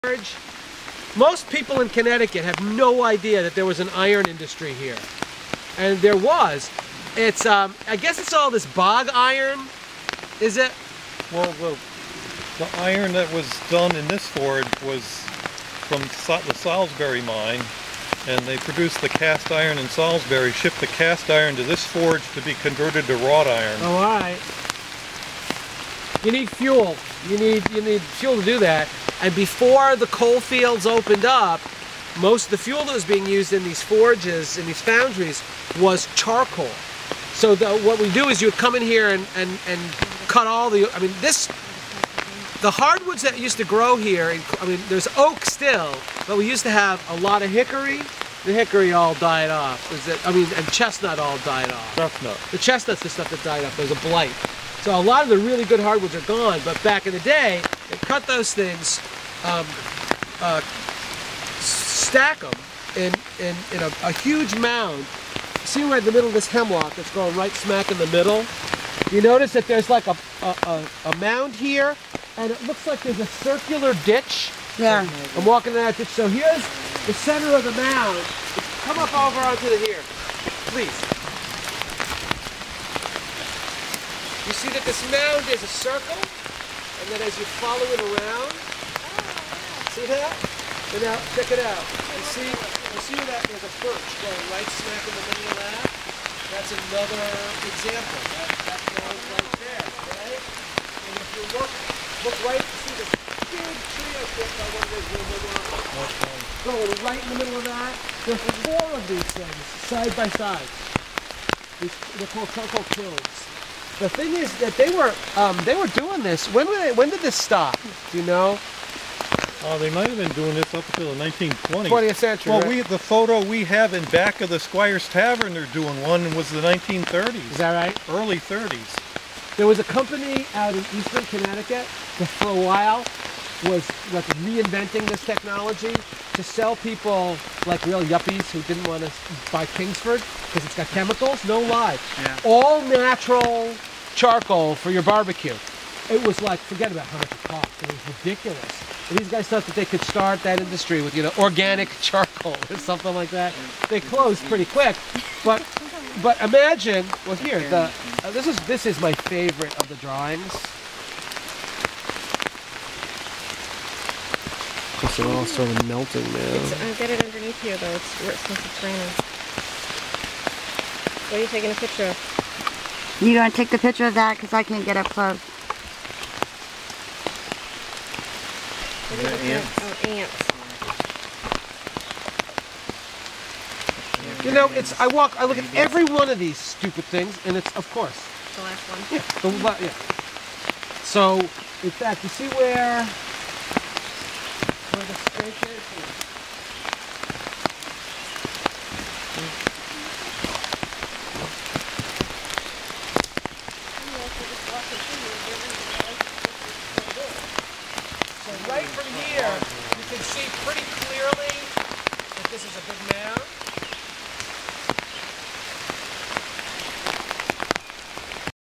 Barkhamsted Lighthouse with Ken Feder - Charcoal Mound 1
7/3/2011 The people that lived here, way back when, used to make and sell Charcoal to the Barkhamsted Town - Here is where the site of it was found - At the Barkhamsted Lighthouse Settlement on Ragged Mountain - Barkhamsted CT. - Ken Feder is showing me where the signs will be put and why they chose these locations - Video taken by Coni Dubois